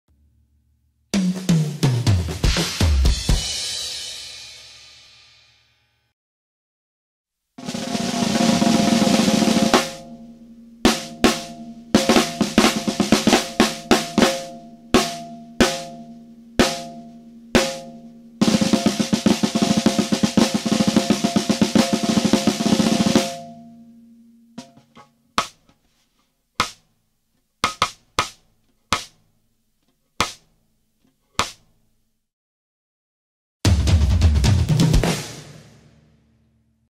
8" X 14" LUDWIG MAPLE SNARE DRUM
Grabado, mezclado y editado en Estudio Epic por Juan Pablo Marroquín

8" X 14" LUDWIG MAPLE SNARE DRUM
Hoops: Triple flanged
Batter head: Remo coated ambassador
Snare side: Evans hazy 200 resonant
Snare wires: 42 strand snare wires